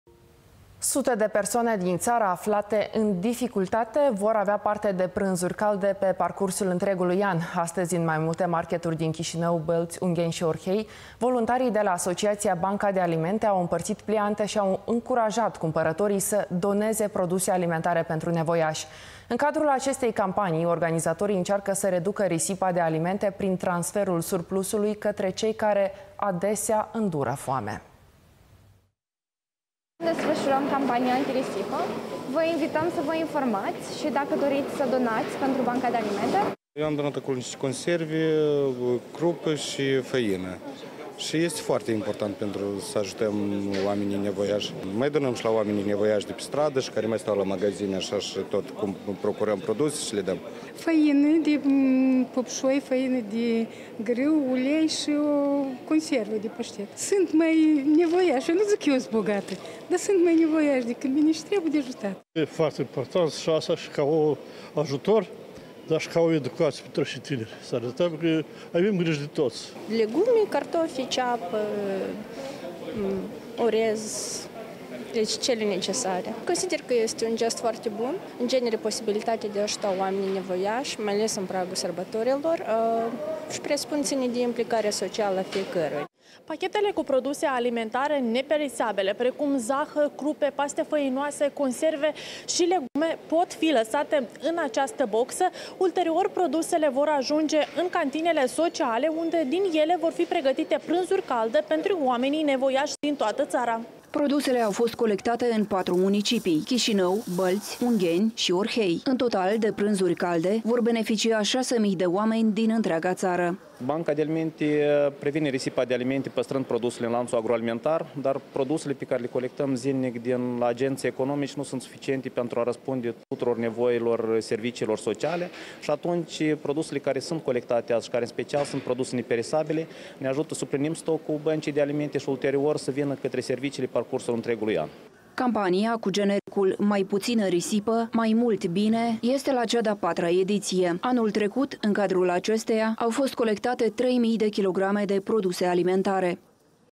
Alimente pentru nevoiaşi
Sute de persoane din ţară, aflate în dificultate, vor avea parte de prânzuri calde pe parcursul întregului an. Astăzi, în mai multe marketuri din Chişinău, Bălţi, Ungheni şi Orhei, voluntarii de la Asociaţia "Banca de Alimente" au împărţit pliante şi au încurajat cumpărătorii să doneze produse alimentare pentru nevoiaşi. În cadrul acestei campanii, organizatorii încearcă să reducă risipa de alimente prin transferul surplusului către cei care adesea îndură foame.